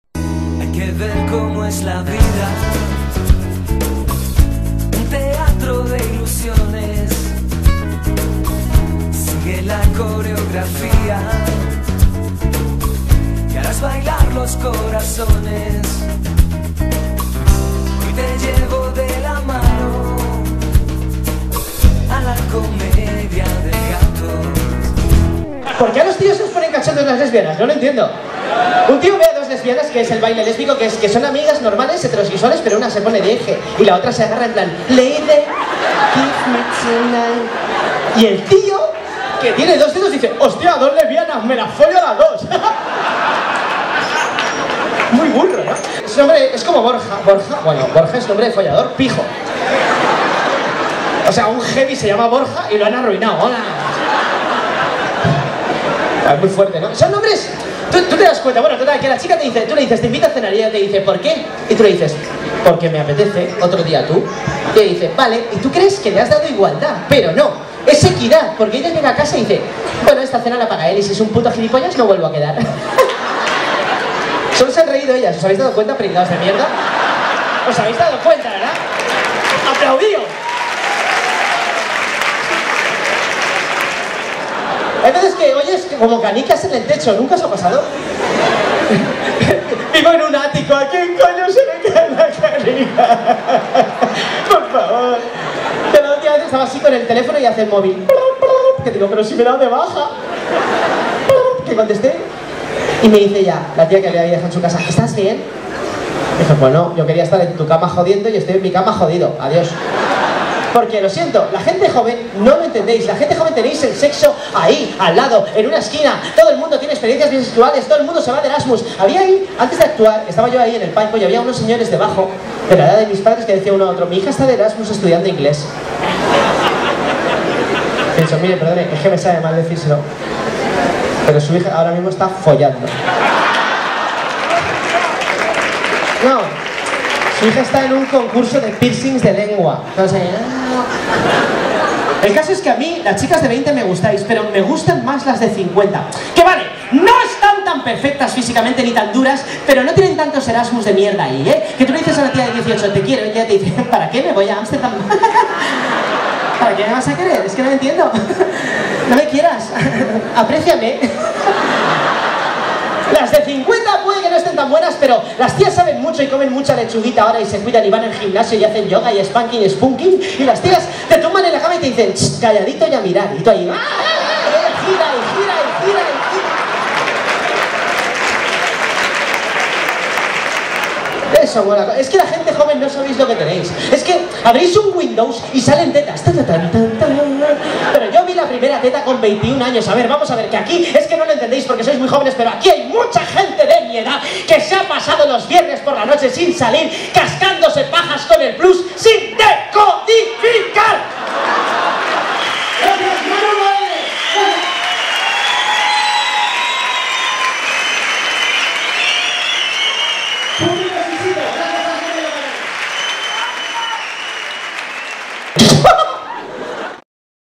Manu Badenes en "Cómicos Uno a Uno"
CONTRATACION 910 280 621 Monologo de Manu Badenes, perteneciente a la gira Comicos uno a uno de Aporarte Producciones